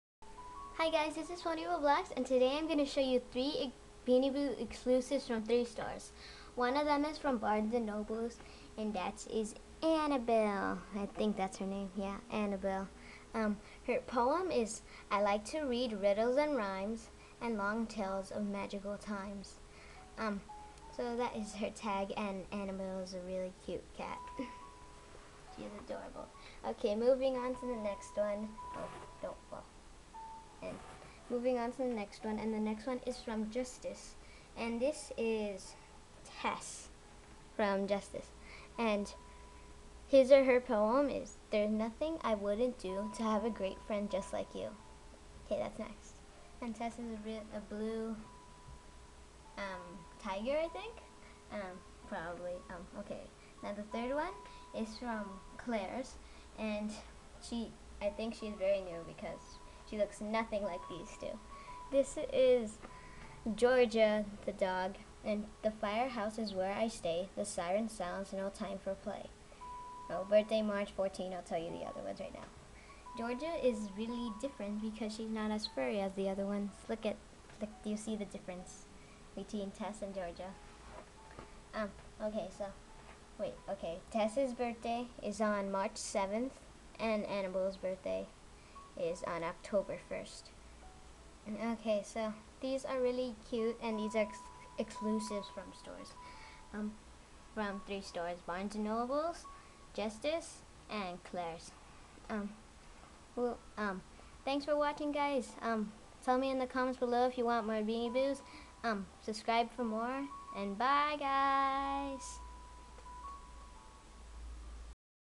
Ty new Beanie Boos Exclusives for my collection
YAAYY!     Barnes and Noble, Justice and Claire's